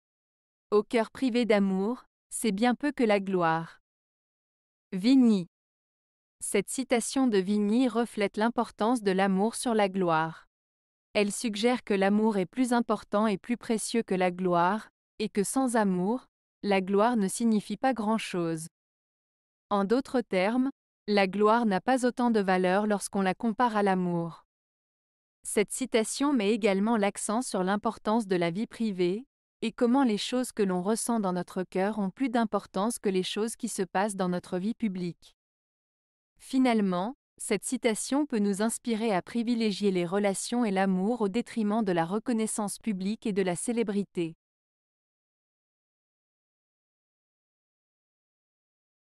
Au coeur privé d'amour, c'est bien peu que la gloire. - Vigny
Cette citation de Vigny reflète l'importance de l'amour sur la gloire. Elle suggère que l'amour est plus important et plus précieux que la gloire, et que sans amour, la gloire ne signifie pas grand chose. En d'autres termes, la gloire n'a pas autant de valeur lorsqu'on la compare à l'amour. Cette citation met également l'accent sur l'importance de la vie privée, et comment les choses que l'on ressent dans notre cœur ont plus d'importance que les choses qui se passent dans notre vie publique. Finalement, cette citation peut nous inspirer à privilégier les relations et l'amour au détriment de la reconnaissance publique et de la célébrité.